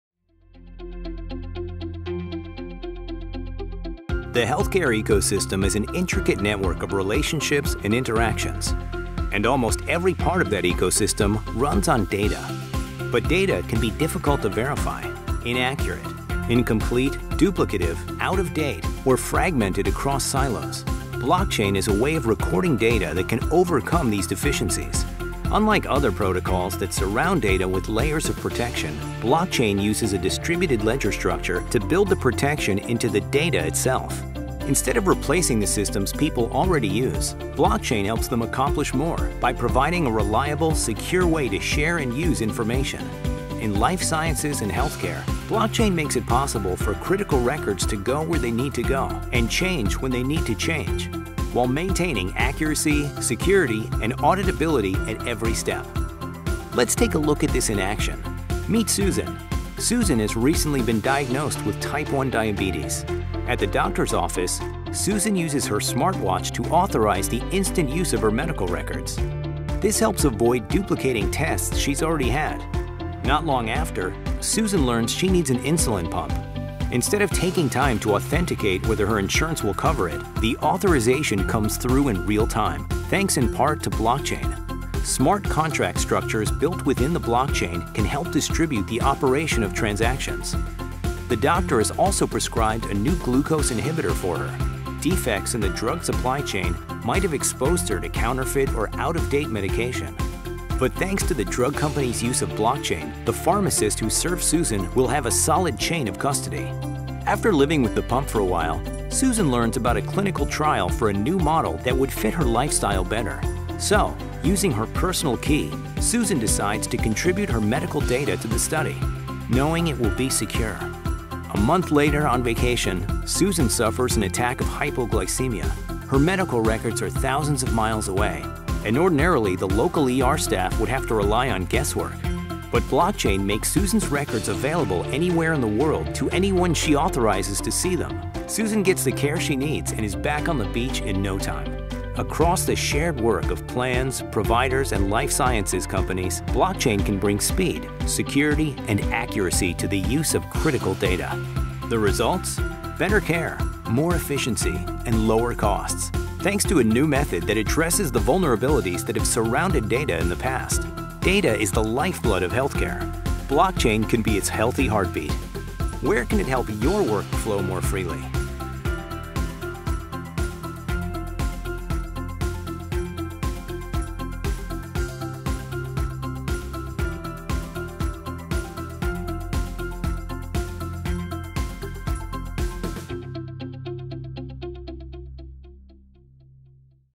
Blockchain in life sciences and health care
Blockchain is changing the life sciences and health care industry in new and unimagined ways. Our video explores how blockchain can help bring speed, security, and accuracy to data sharing in the life sciences and health care industry.